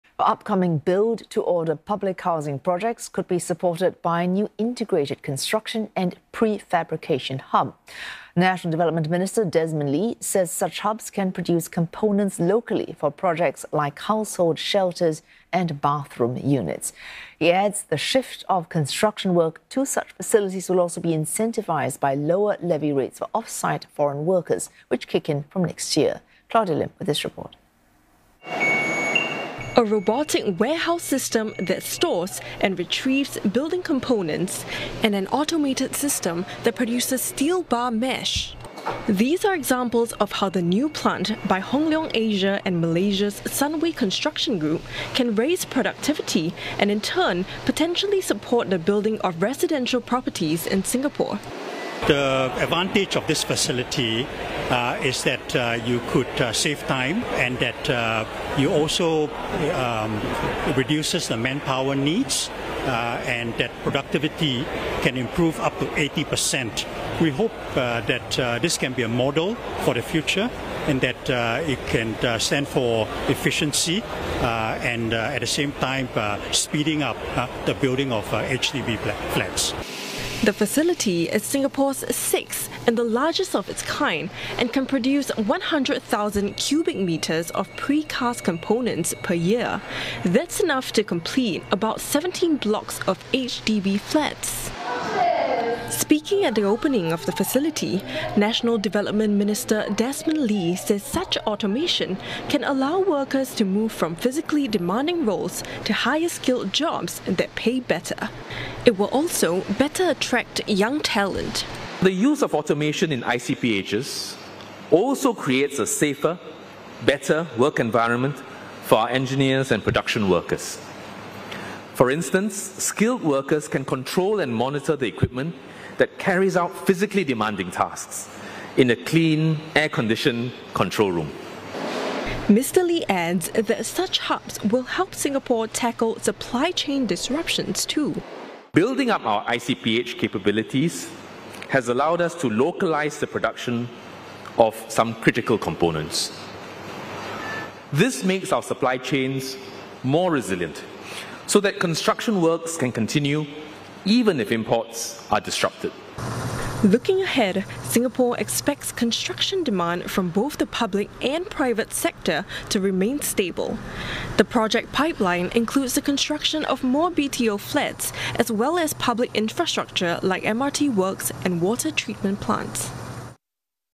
New construction and prefabrication hub can support upcoming BTO projects: Desmond Lee
Upcoming Build-to-Order public housing projects could be supported by a new integrated construction and prefabrication hub. National Development Minister Desmond Lee said such hubs can produce components locally for projects like household shelters and bathroom units. He added that the shift of construction work to such facilities will also be incentivised by lower levy rates for off-site foreign workers, which kick in from next year.